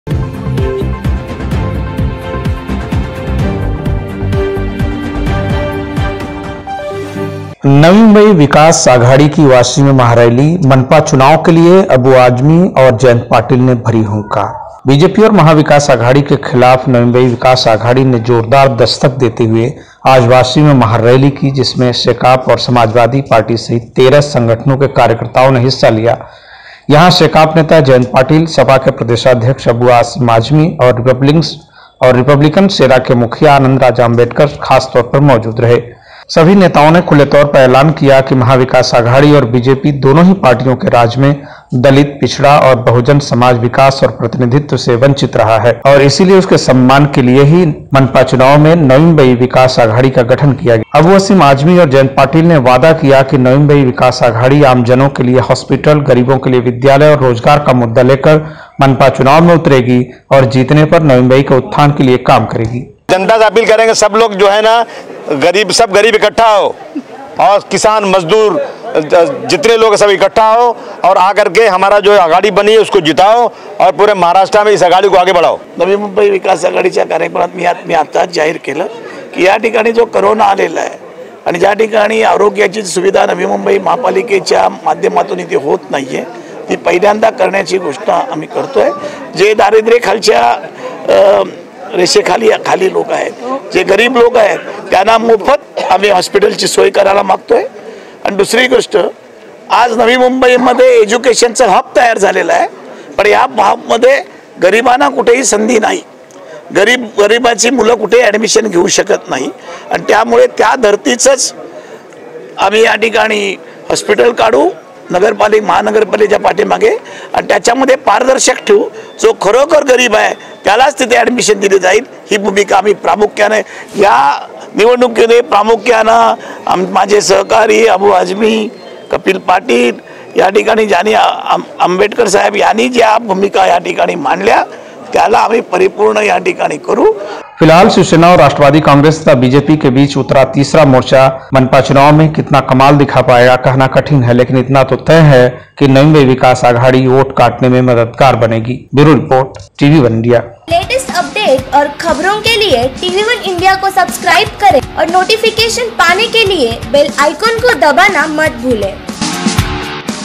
नवी मुंबई विकास आघाड़ी की वाशी में महारैली । अबू आजमी और जयंत पाटिल ने भरी हुंकार
बीजेपी और महाविकास आघाड़ी के खिलाफ नवी मुम्बई विकास आघाड़ी ने जोरदार दस्तक देते हुए आज वाशी में महारैली की जिसमे शेकाप और समाजवादी सहित 13 संगठनों के कार्यकर्ताओं ने हिस्सा लिया. यहां शेकाप नेता जयंत पाटिल, सपा के प्रदेशाध्यक्ष अबू आसिम आजमी, और रिपब्लिकन सेना के मुखिया आनंदराज आंबेडकर खास तौर पर मौजूद रहे.